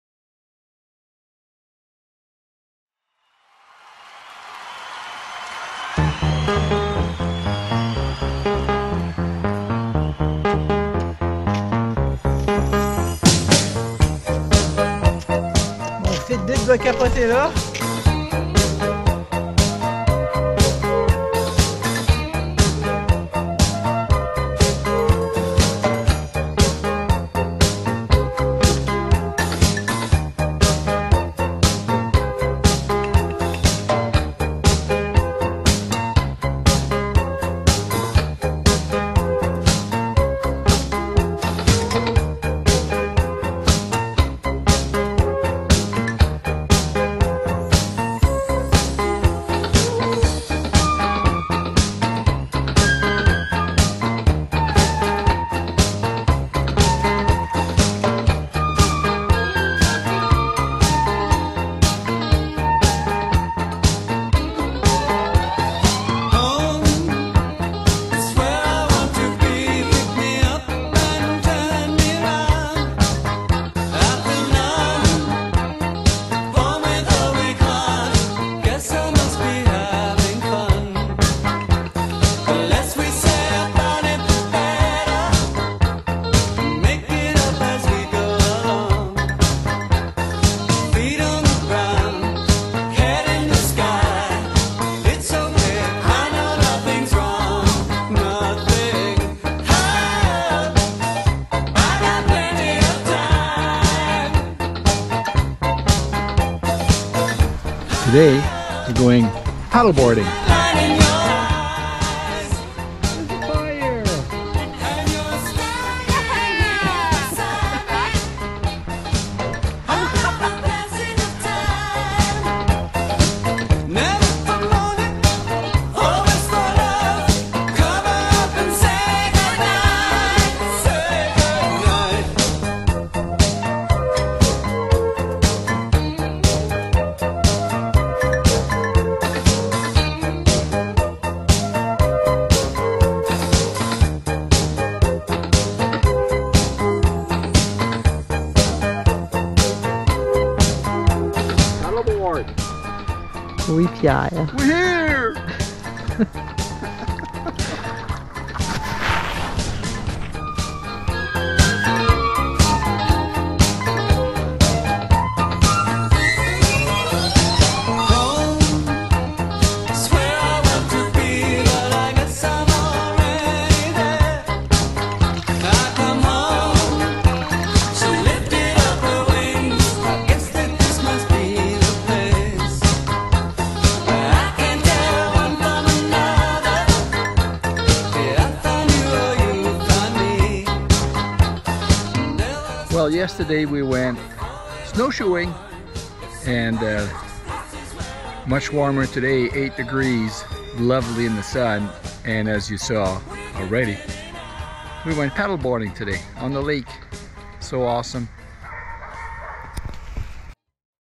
The Place We Call Home
Last weekend activities  Saturday, we were snowshoeing at Teapot Hill. Sunday, we were paddle boarding at Cultus Lake. Not bad not bad for this time of year. And, not bad for creating a sense of adventure in our own backyard.